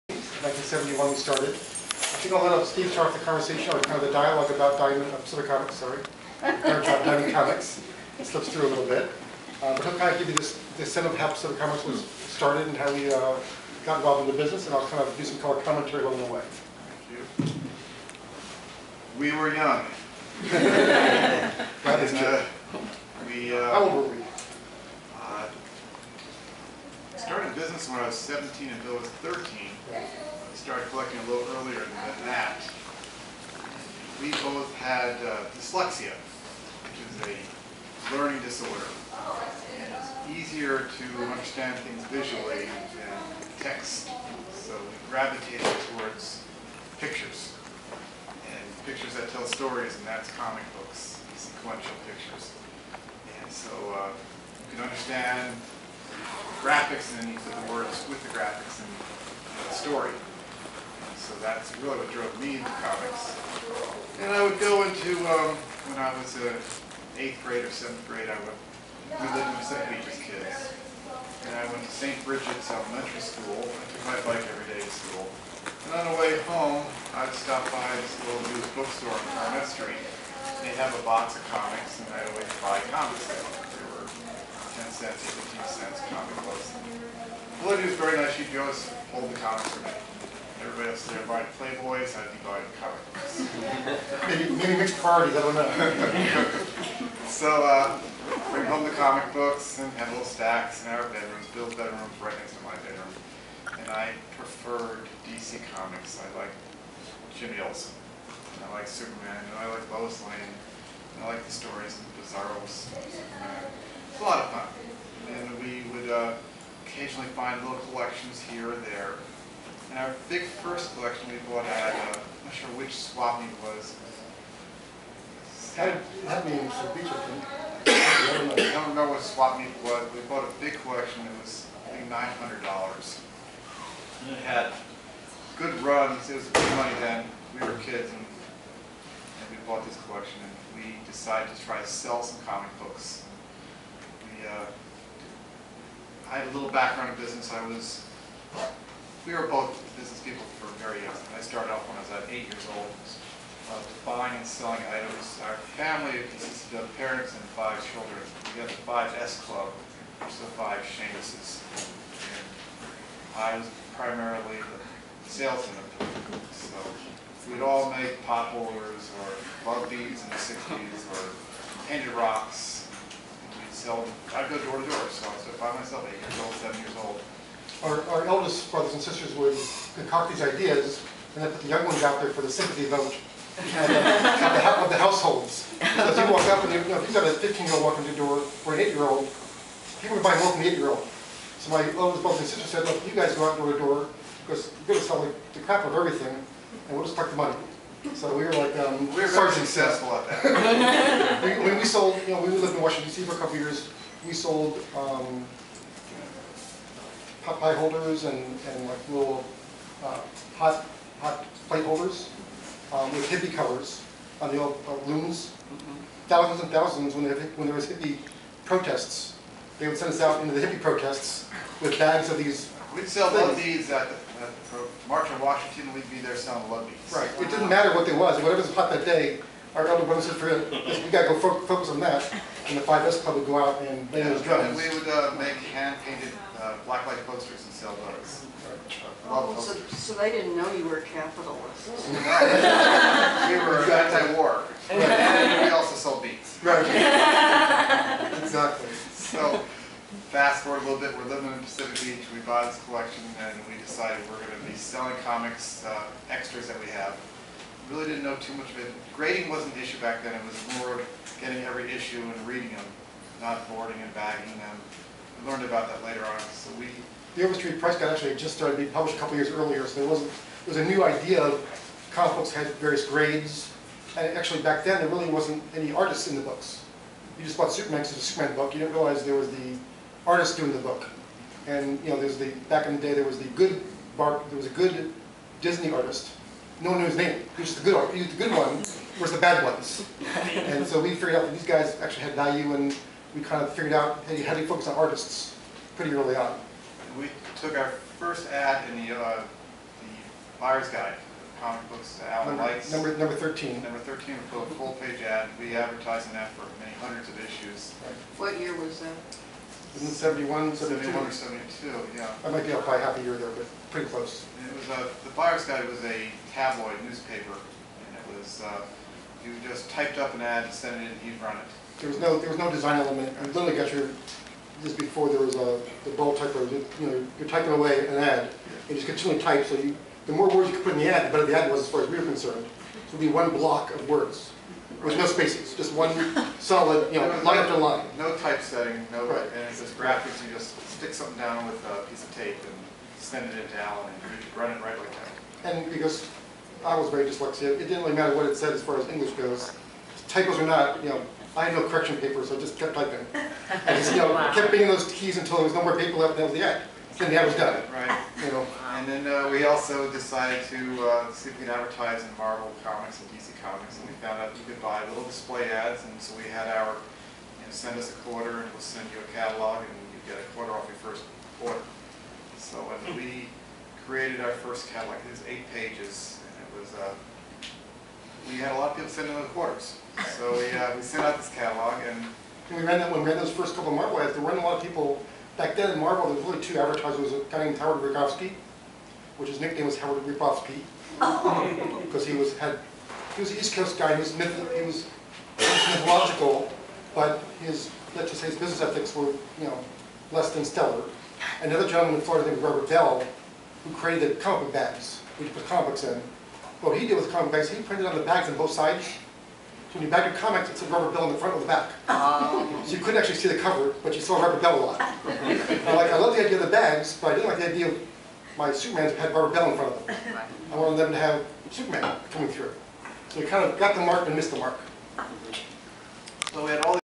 2013 San Diego Comic Fest - Schanes Brothers Early History!
2013 San Diego Comic Fest - Schanes Brothers Early History!  Start of the panel held on Saturday, October 5th, at the Comic Fest.  Steve Schanes (left) and Bill (William) Schanes start the panel in this video.